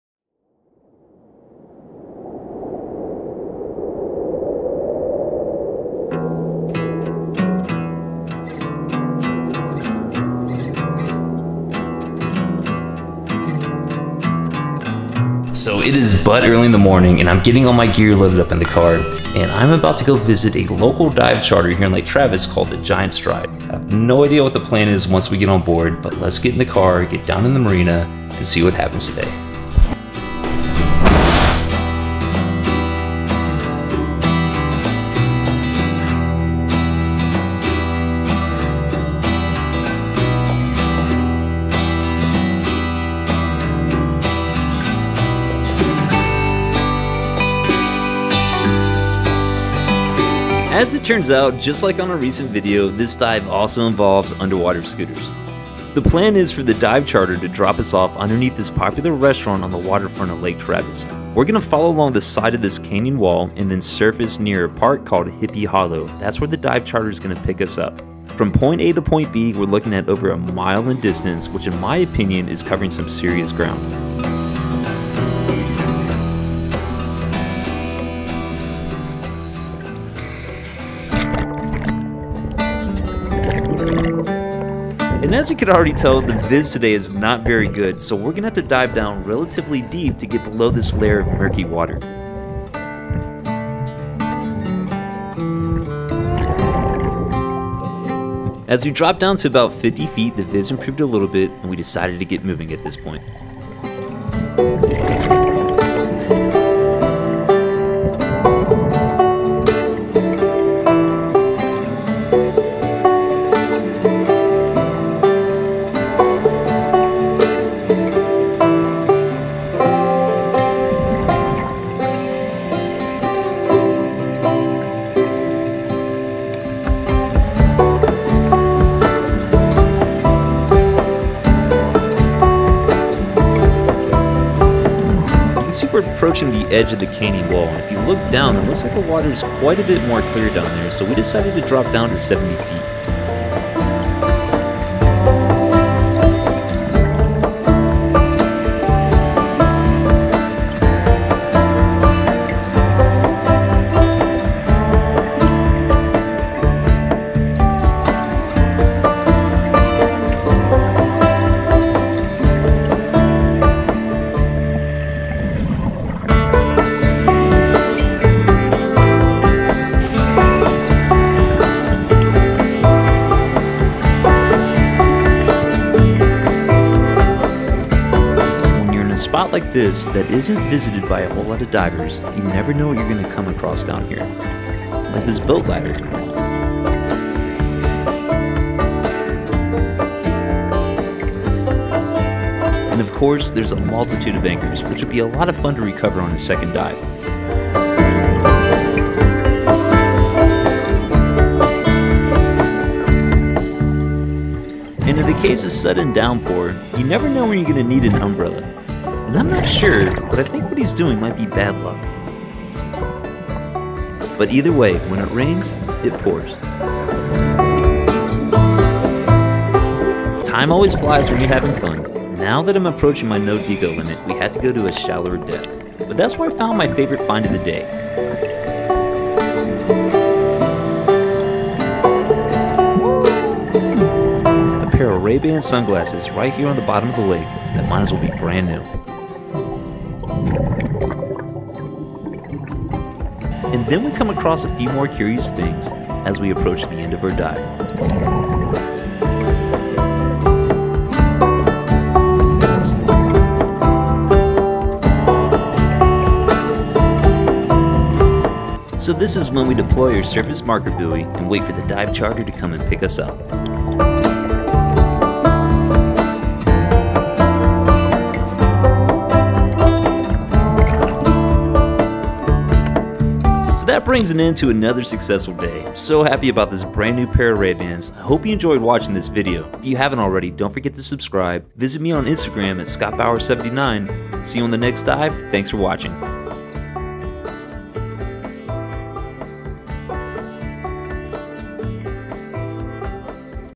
1 Mile Underwater Scooter Ride & Soggy New Pair of Ray-Ban Sunglasses
We rode these underwater scooters along the Oasis Wall in Lake Travis for over a mile, took a ride on a local dive charter (The Giants Stride) and found a soggy brand new pair of Ray-Ban Sunglasses